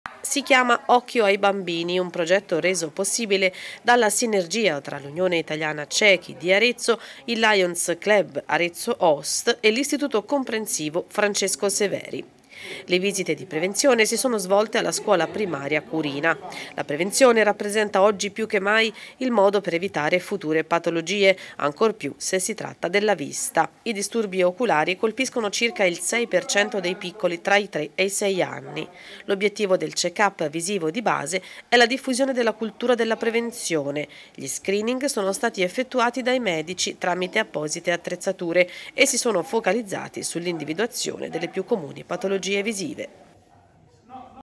“Occhio ai bambini”, prevenzione delle anomalie visive a scuola
Si chiana “Occhio ai bambini”, un progetto reso possibile dalla sinergia tra Unione Italiana Ciechi e Ipovedenti di Arezzo, IAPB Italia Onlus (Agenzia Internazionale per la Prevenzione della Cecità) e il supporto del Lions Club Arezzo Host e che si è svolto presso l’IC Francesco Severi alla Scuola Primaria della Curina.

La prevenzione rappresenta, oggi più che mai, il modo per evitare future patologie in particolare quando si tratta della vista.
I disturbi oculari colpiscono circa il 6% dei piccoli tra i 3 e i 6 anni.
L’obiettivo del check up visivo di base è la diffusione della cultura della prevenzione.
Gli screening sono stati effettuati dai medici, tramite apposite attrezzature e si sono focalizzati sull’individuazione delle più comuni patologie visive.